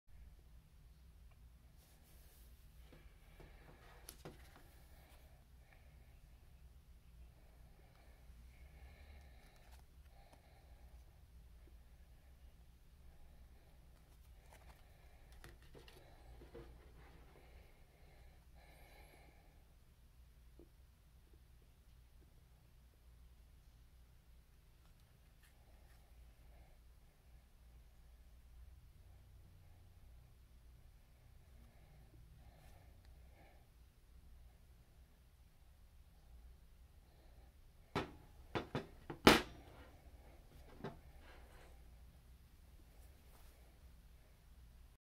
1988-89 OPC Brett Hull rookie card vs. the Highland mint Topps 4.25 oz. fine silver .999 Brett Hull
Note the highland mint released the silver card in years later but it's a stunning piece of silver